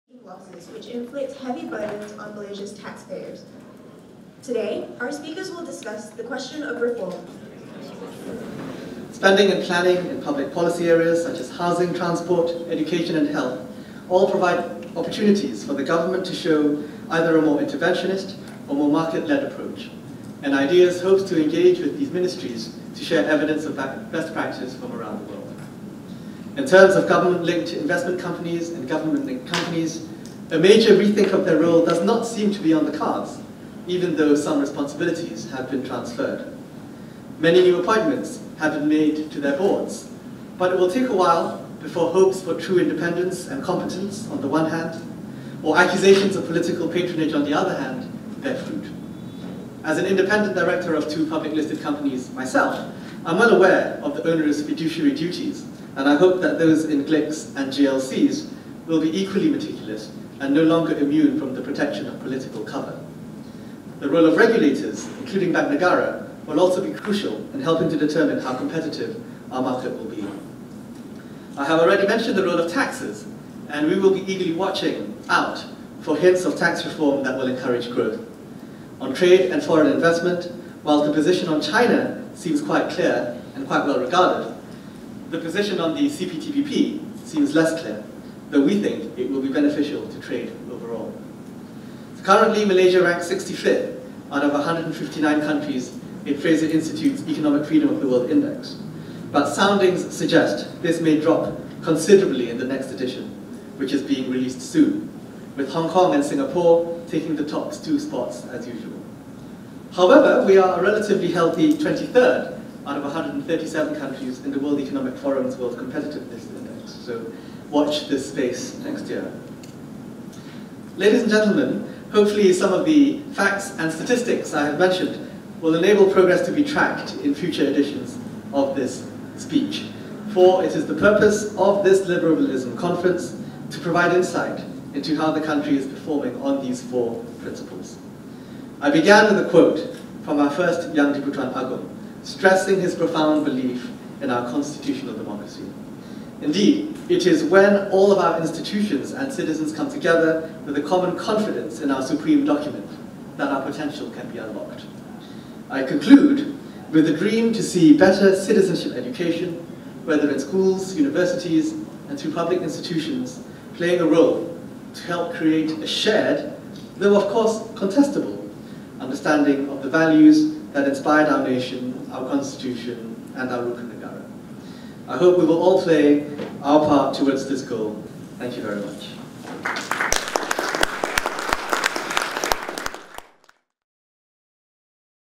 IDEAS: Positive signs for better governance in GLCs
In his speech at the third Liberalism Conference held in Kuala Lumpur on Saturday, Institute for Democracy and Economic Affairs (IDEAS) president Tunku Zain Al-'Abidin said the recent reappointments of top executives in the government-linked companies (GLCs) are a good sign but it will take a while before true competence and independence in the companies will be realised.